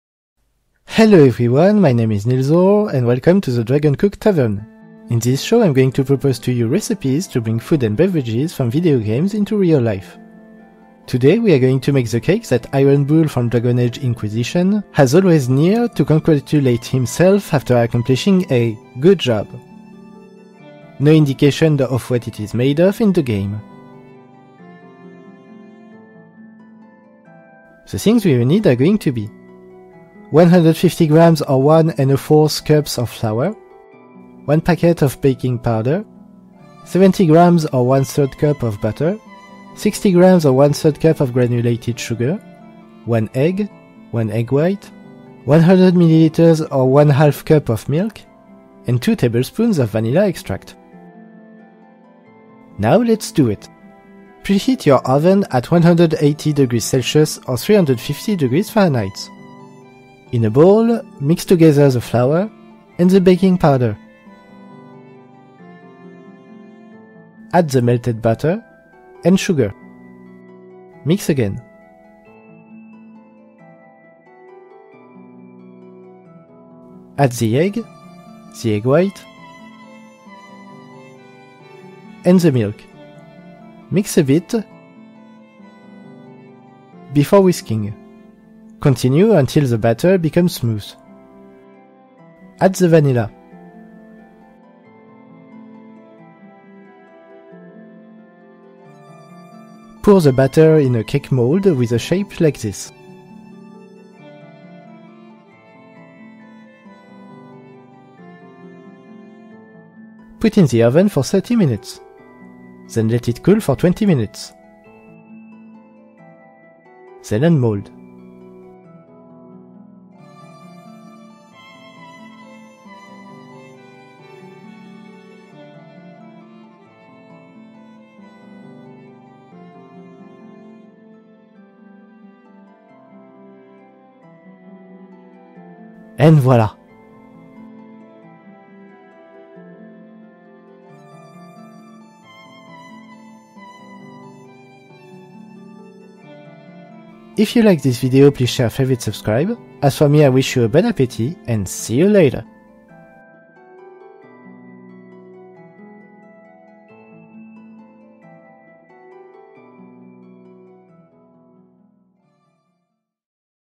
Iron Bull's "Good Job" Cake - Dragon Age - [The Dragon Cook Tavern]
"The Dragon Cook Tavern" is a cooking show aiming to bring video games food and beverages into real life.

Subtitles available in English and French.

Episode content
"Iron Bull's cake" from Dragon Age: Inquisition

Miscellaneous
All videos and images belong to their righfult owners, no copyright infrightment intended

Edited with Sony Vegas 12 and Adobe After Effect CS5